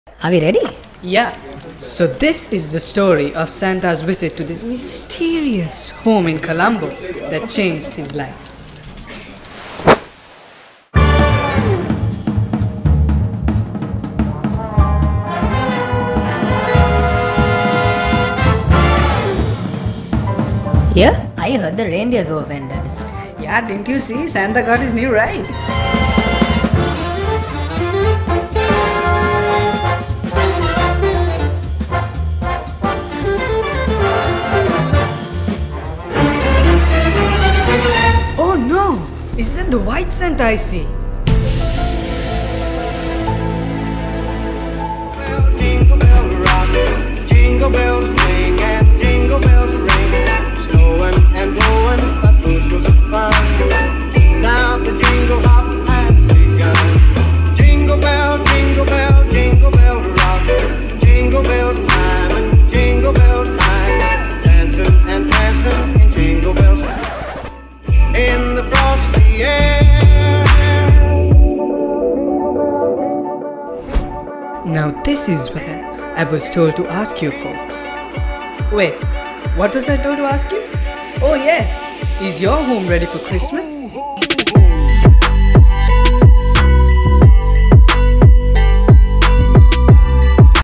Christmas in Colombo - Part 1
Christmas is almost here! Is your home ready for Christmas?

This is the story of Santa's visit to a mysterious home in Colombo that changed his Life!